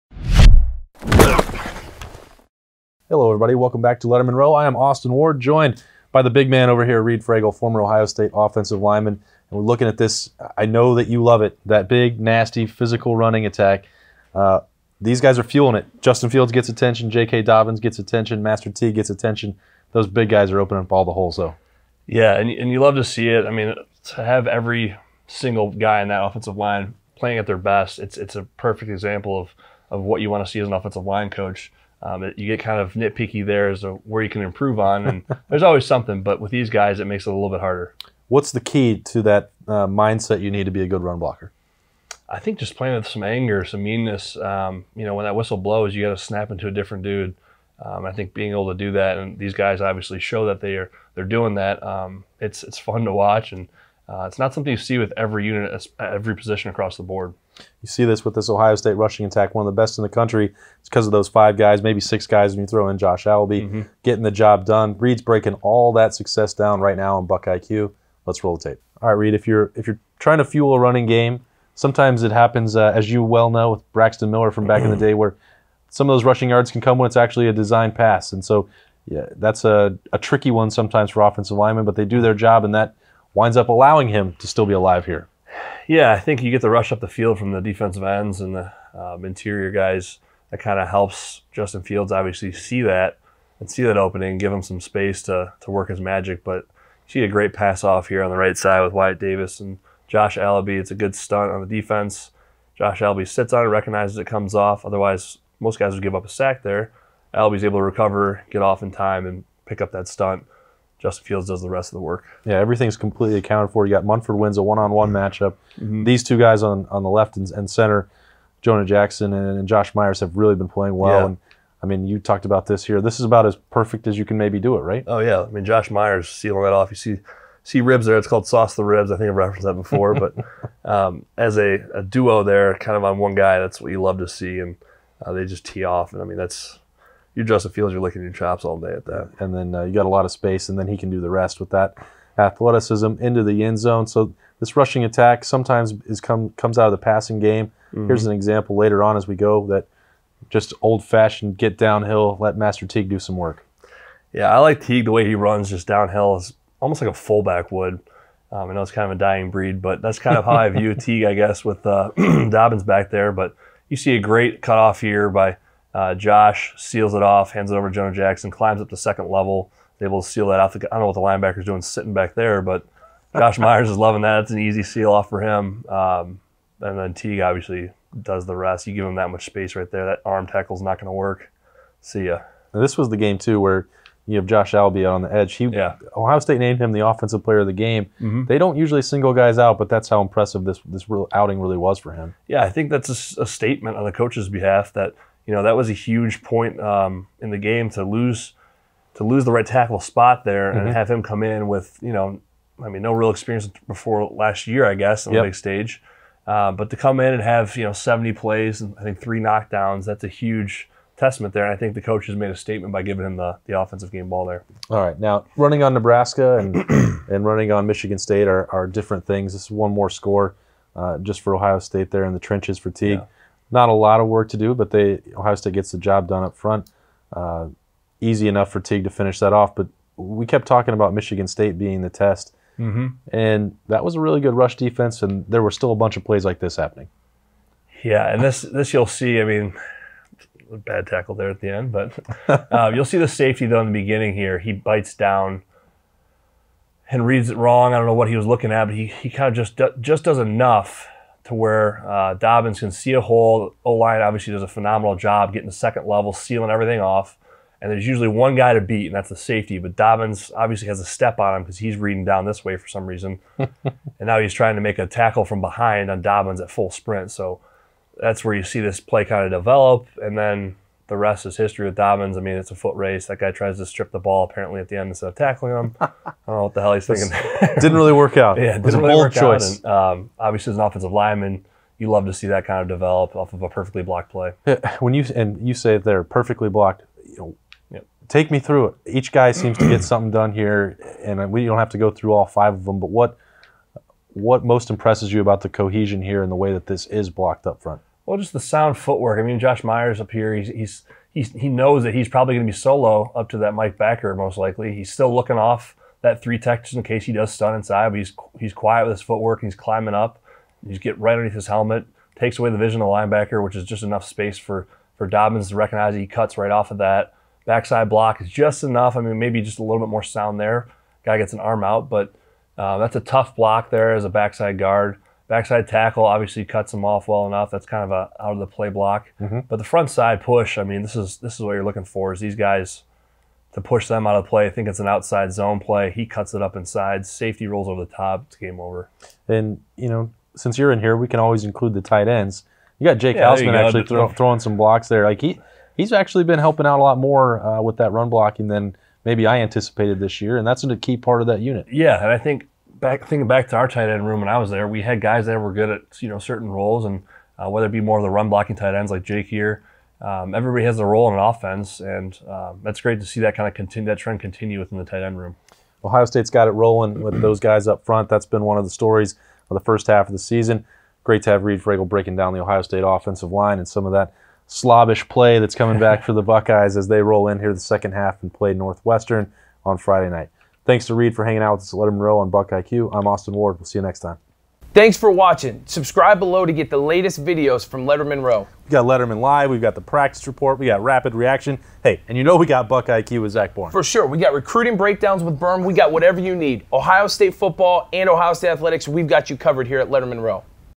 BuckIQ: Tough, smart interior linemen springing Ohio State rushing attack
What has helped Josh Myers and the other first-time starters on the Ohio State offensive line thrive so far this season? Can the powerful rushing attack get even better as the season progresses? Where are the areas the Buckeyes might still be able to grow down the stretch? Former Ohio State offensive lineman Reid Fragel put on the film to answer those questions in the latest episode of BuckIQ.

Roll the tape!